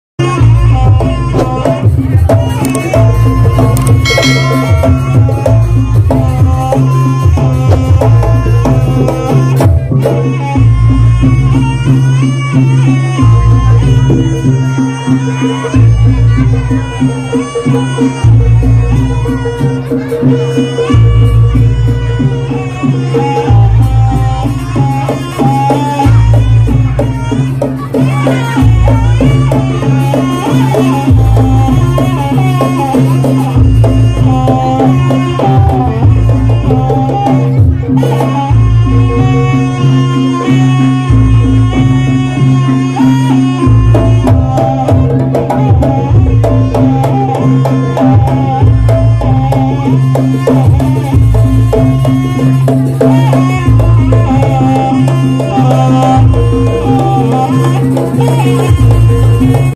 SENG SUNAT MANUKKE DI DAMONI, SUK BEN GANAS🤣 Jaranan Mustiko Dewo
Seni Jaranan Campursari
Mustiko Dewo Petahunan Jajag
Gambiran Banyuwangi Jawatimur